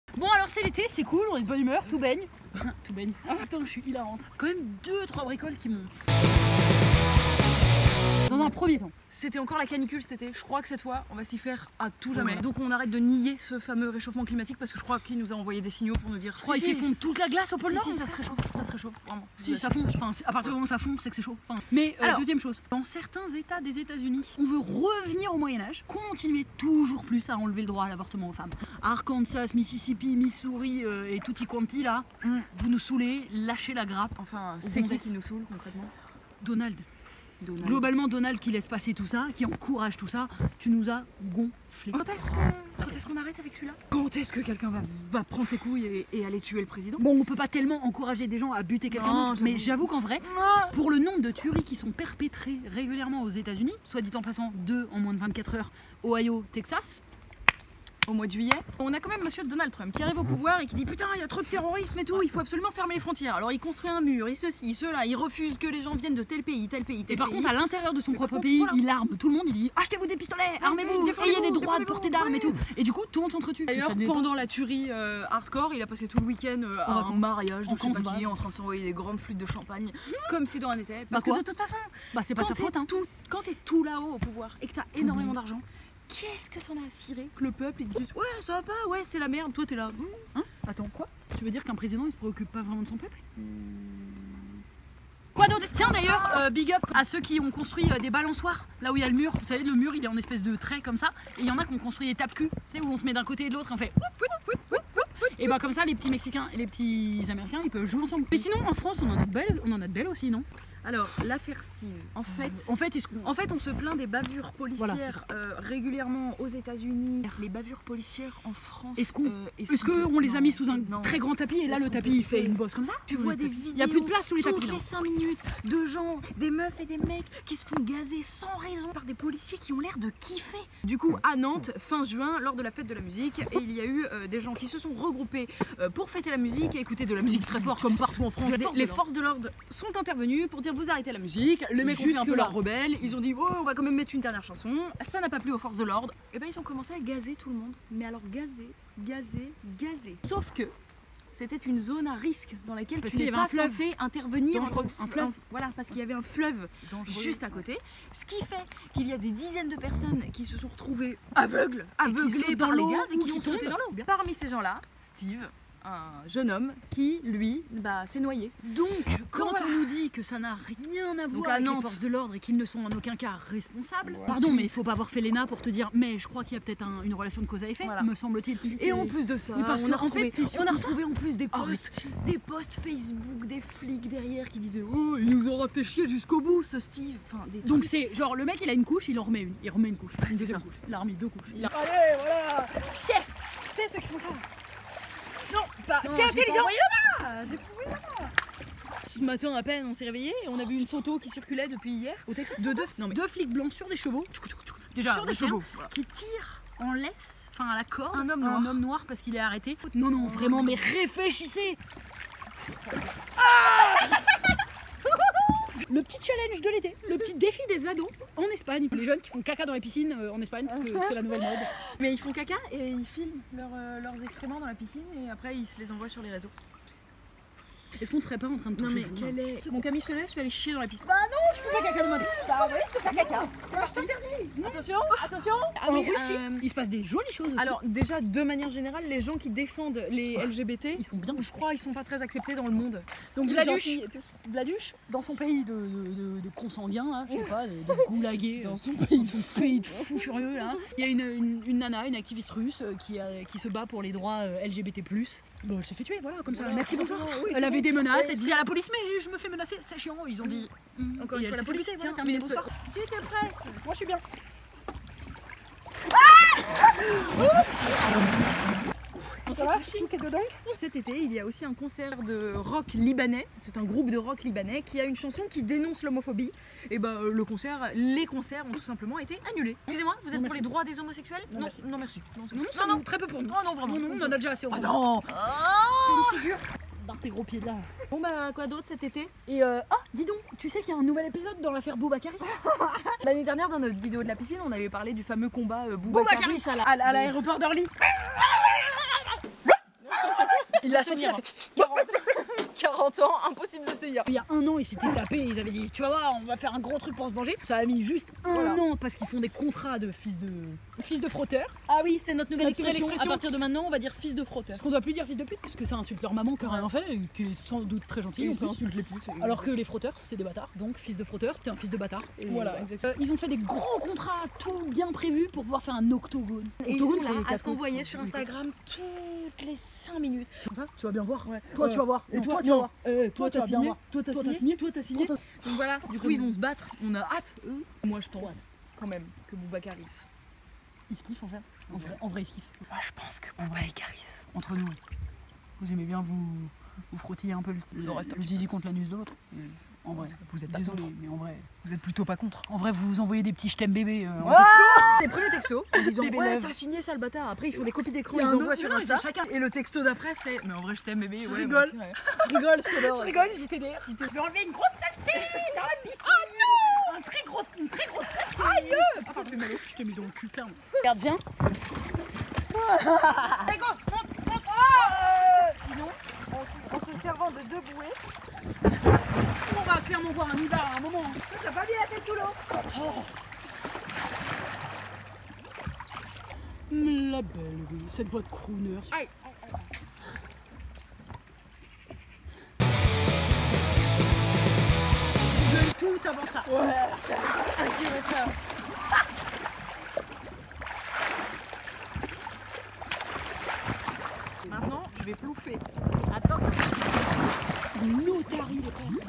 Les actus dans la piscine - Août 2019
CE QUE CES FILLES FONT DANS CETTE PISCINE EST ENCORE PLUS INCROYABLE QUE LA PREMIÈRE FOIS, ÇA VA ENCORE PLUS VOUS SURPRENDRE WAAAAN

Les lois anti-avortement aux US, les bavures policières, l'affaire Steeve, le combat Booba-Kaaris... etc. On commente les actus de l'été dans la PISTOCHE.

L'été c'est fini, on espère que vous avez passé de belles vacances, et que cette vidéo de la rentrée vous plaira :)

Commentez, partagez, likez

💁Sur Snapchat
camilletjustine